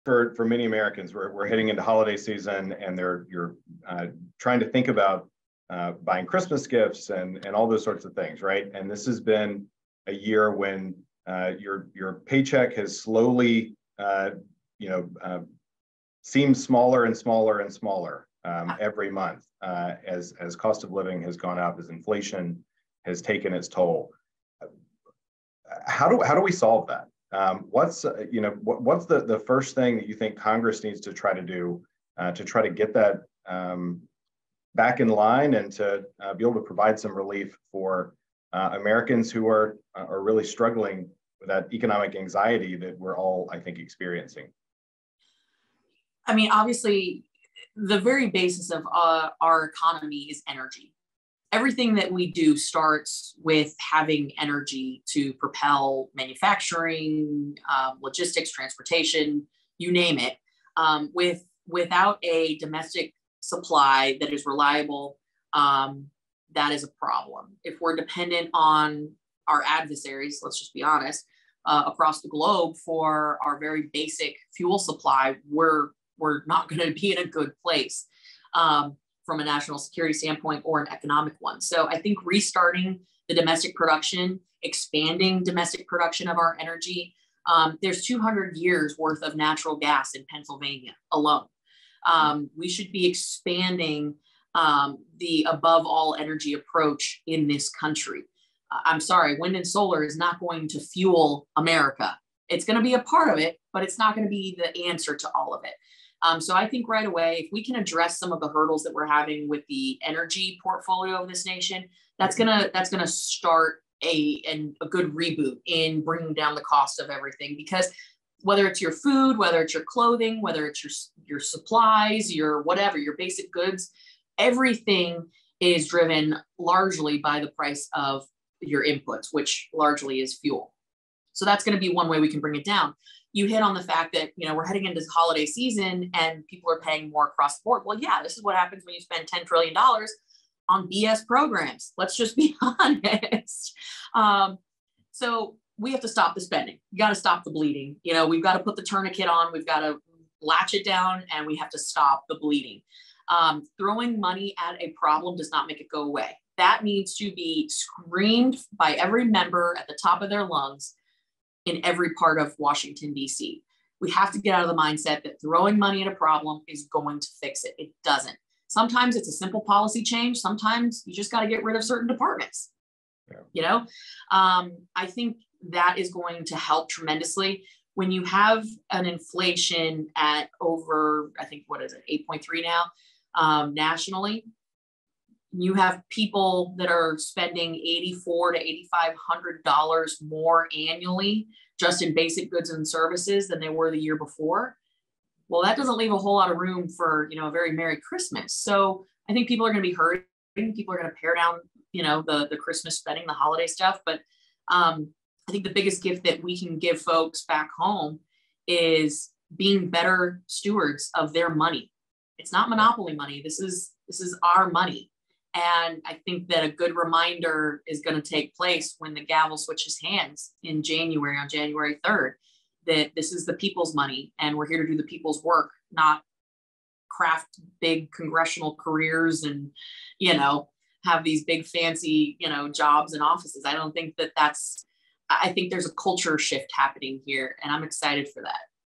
Reining in Big Government in DC
In her first two years as a representative in Congress, Cammack has become well acquainted with policies that won’t solve the issues of everyday Americans. As she explained:

“Throwing money at a problem does not make it go away. That needs to be screamed by every member at the top of their lungs in every part of Washington, DC. We have to get out of the mindset that throwing money at a problem is going to fix it.”

However, instead of yelling from the rooftops about the dangers of spending, many of her colleagues in Congress have spent the last two years expanding the size and scope of government.

“We create all these programs that are designed really, in my opinion, to keep people dependent on government programs. And that in turn turns to the scope, which is the everyday involvement of government in our lives, from everything from a mandate to the regulatory environment.”

Visit the AFP Action Center take action on issues like this one